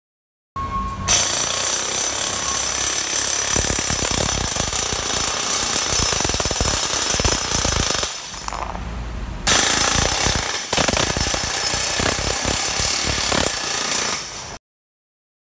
Rhino Hammer from Slatpro, LLC
The Rhino Hammer makes removing tabbed parts much easier after they are cut. It uses shop air to create impact vibration that breaks the tab loose, leaving the finished parts in good condition. A unique polyurethane tip handles the air pressure necessary for removal of parts. Available with either standard 1.5” diameter tip or small .75” tip.